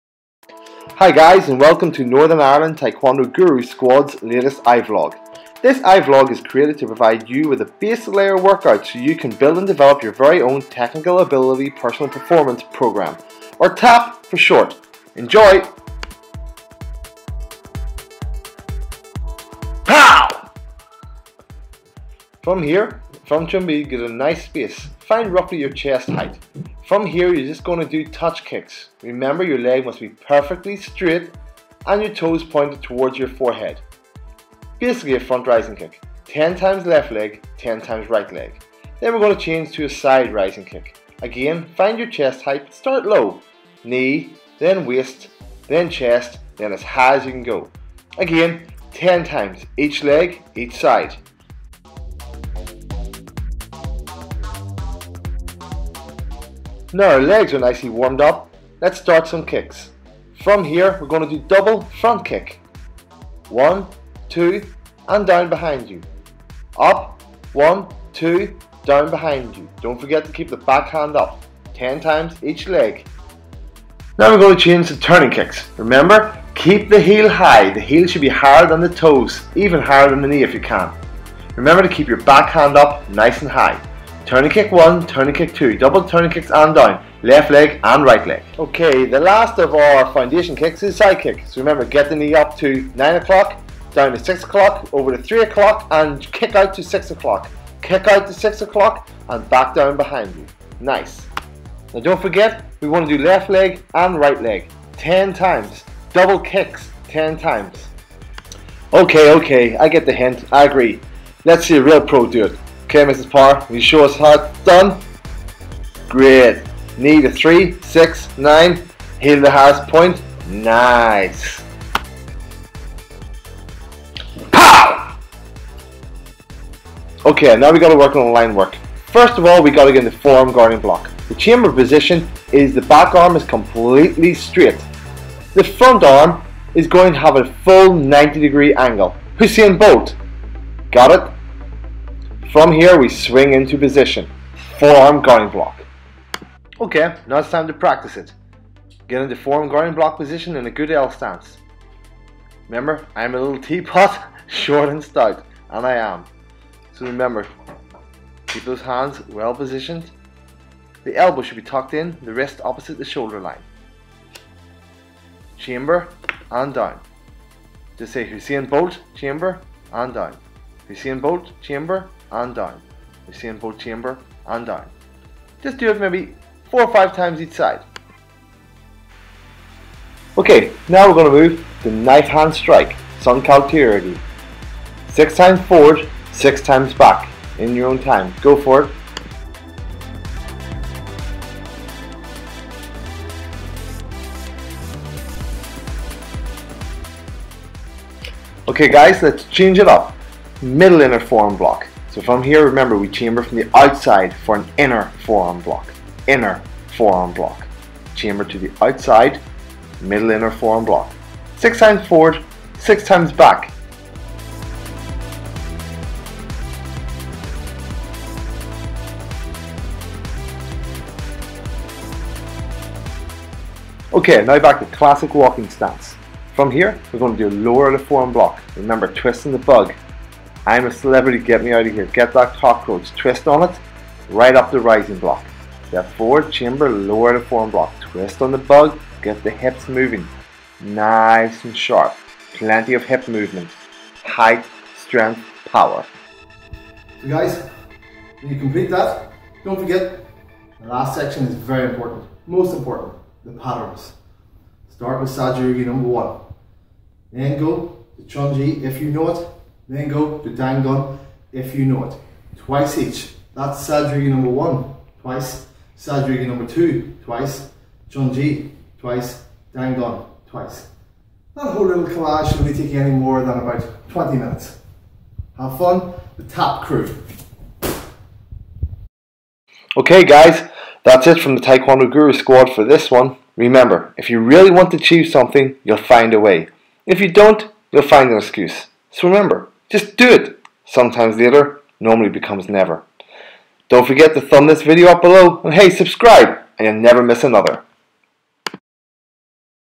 TAGB Nitkd Guru Squad iVlog 3 - TAPP Programme
Your "Technical Ability Personal Performance Programme" (TAPP) to work on at home!
Hey! Thumb it up!